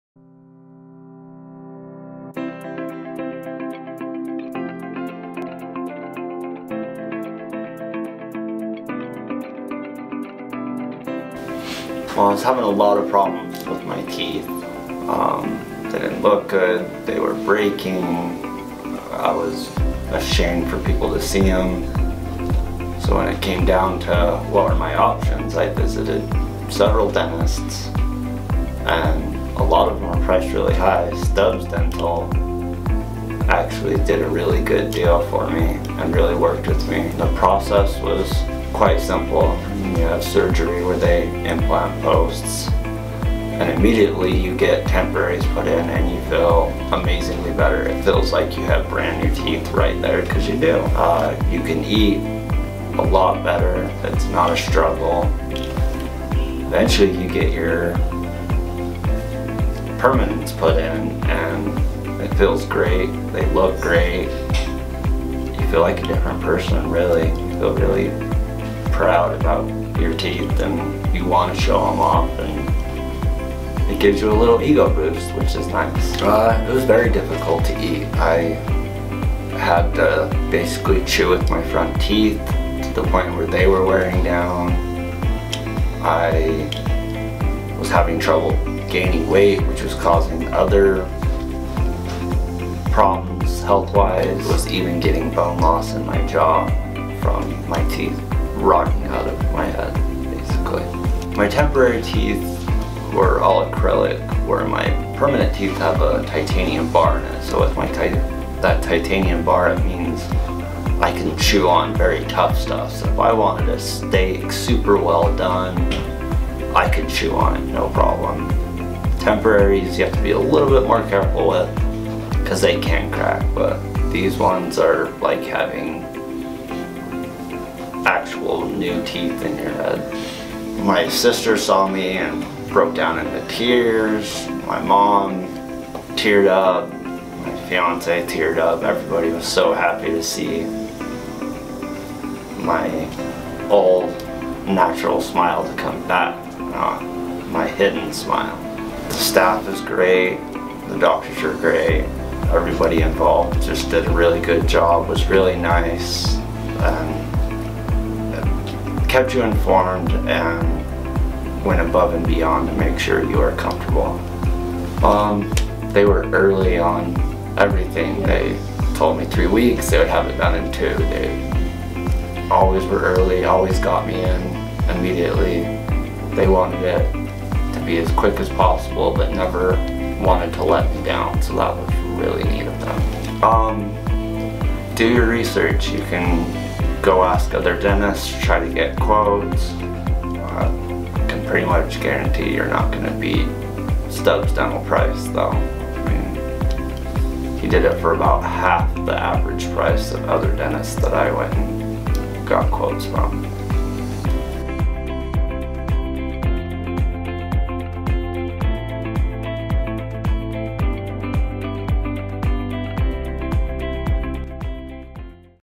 Zach All-On-4 Fixed Hybrid Denture Testimonal
See what Zach has to say about his brand new smile!
"My family became so emotional when they saw my natural smile again." -Zach